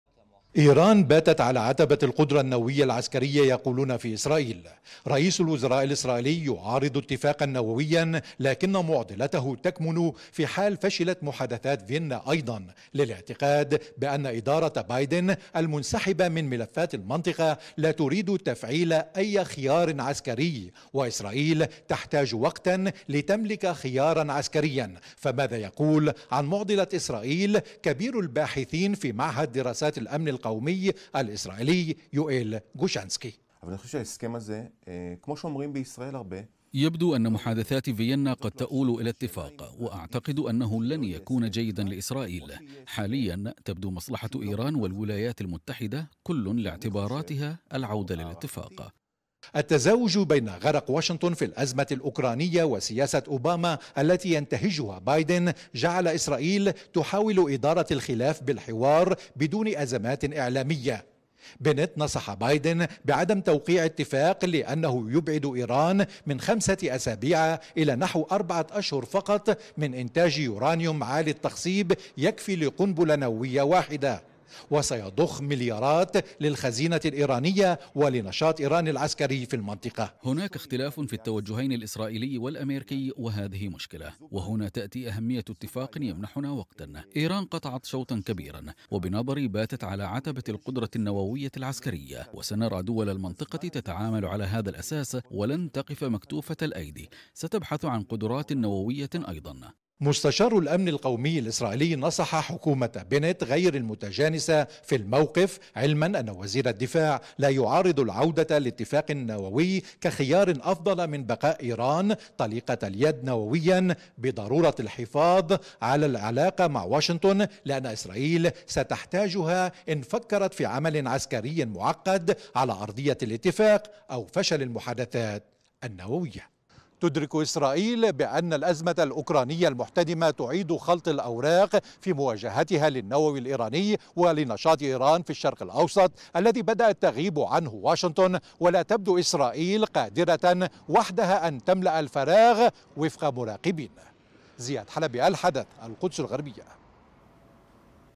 خطة إسرائيل للتعامل مع طهران مع قرب صنع قنبلة نووية
إسرائيل ترتب أوراقها حال فشل مباحثات فيينا وقرب طهران من صنع قنبلة نووية.. كبير الباحثين في معهد الأمن القومي الإسرائيلي: تل_أبيب تدير خلافها مع واشنطن بالحوار